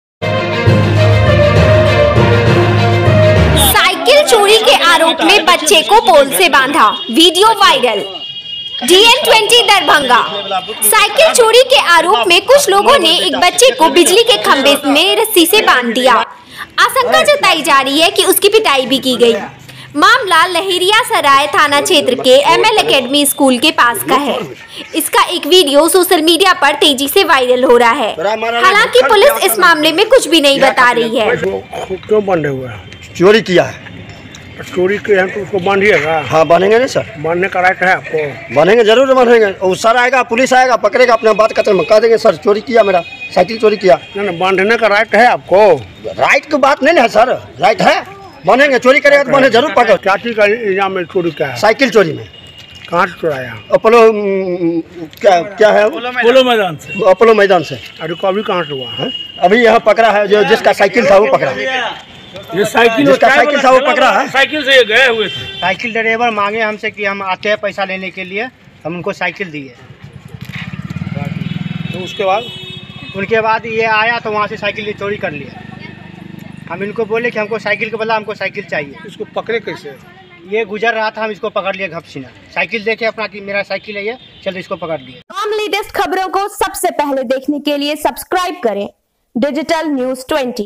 साइकिल चोरी के आरोप में बच्चे को पोल से बांधा, वीडियो वायरल
DN20, Darbhanga : साइकिल चोरी के आरोप में कुछ लोगों ने एक बच्चे को बिजली के खंभे में रस्सी से बांध दिया। आशंका जताई जा रही है कि उसकी पिटाई भी की गई। मामला लहेरियासराय थाना क्षेत्र के एमएल एकेडमी स्कूल के पास का है। इसका एक वीडियो सोशल मीडिया पर तेजी से वायरल हो रहा है। हालांकि पुलिस इस मामले में कुछ भी नहीं बता रही है।
 वायरल वीडियो में दिख रहा है कि एक बच्चे को बिजसी के खंभे से बांधा गया है। वहां कई लोग तमाशबीन बने खड़े हैं। एक व्यक्ति उस बच्चे पर चोरी का आरोप लगा कर उसे गालियां दे रहा है। इस बीच किसी ने घटना का वीडियो बना लिया और वायरल कर दिया।

:21
02igshid:1 2m6qc65f6r02
20/pref:bookmarks